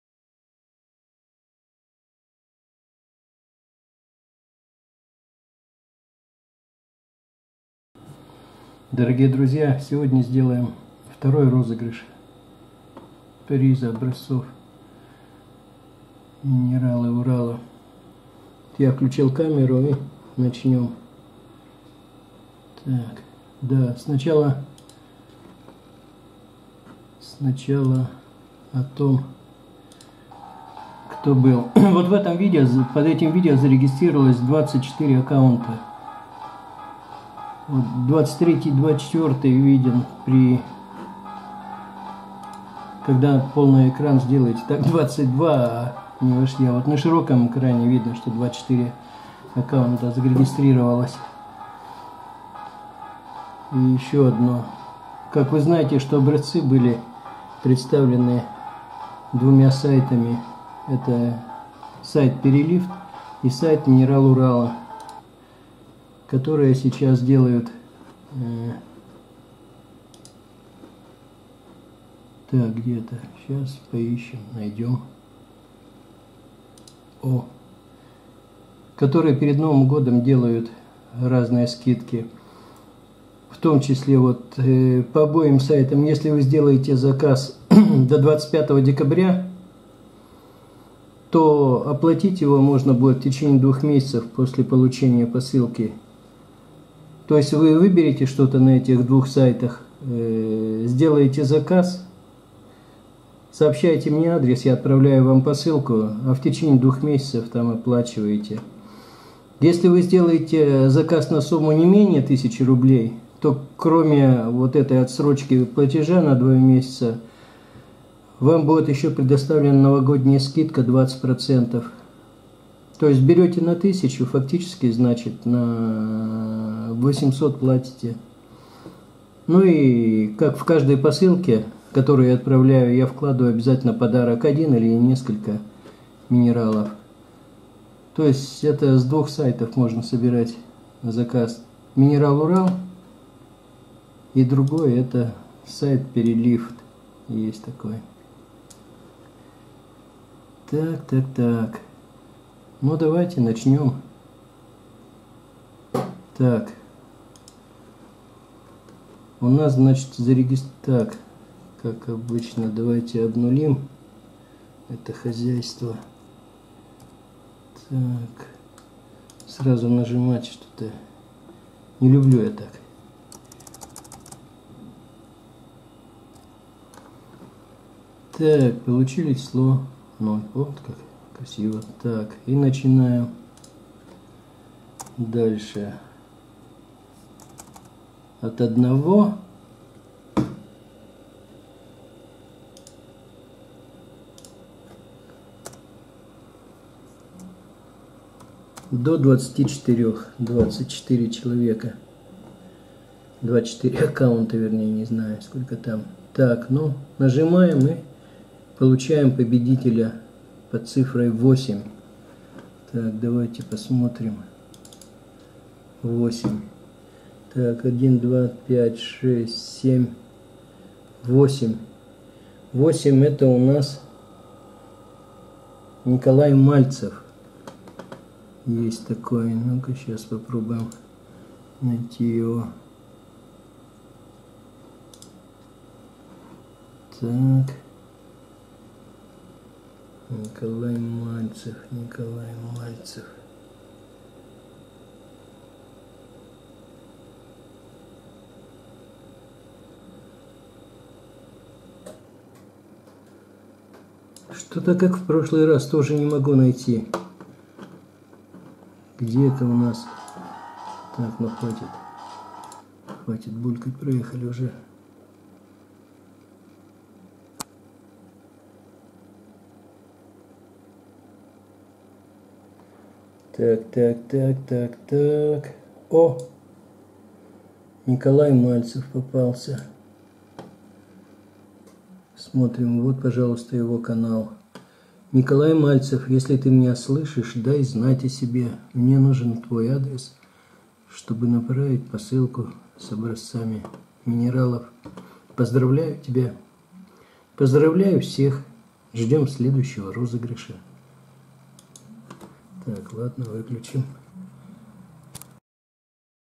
Итоги второго розыгрыша приза Минералы Урала
Поздравляю победителя! Образцы для приза  предоставлены   сайтами  MINERALURAL и PERELIVT.
Произведение «Итоги второго розыгрыша приза Минералы Урала» созданное автором по имени Владимир Маковецкий, публикуется на условиях лицензии Creative Commons «Attribution» («Атрибуция») 4.0 Всемирная.